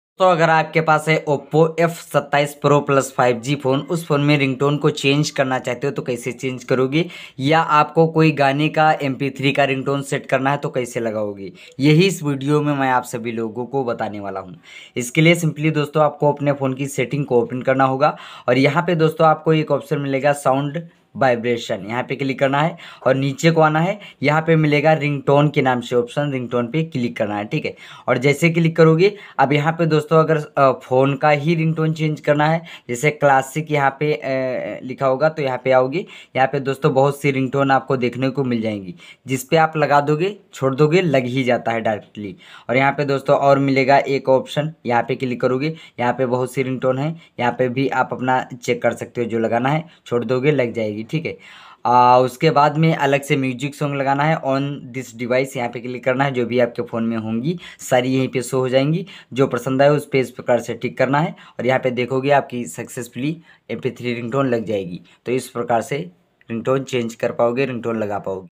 how to change ringtone in oppo f27 pro plus 5g / oppo f27 pro plus me ringtone kaise lagaye
Hello dosto aaj ki is video mein humne aapko bataya oppo f27 pro plus 5g me ringtone chenge kaise kiya jata hai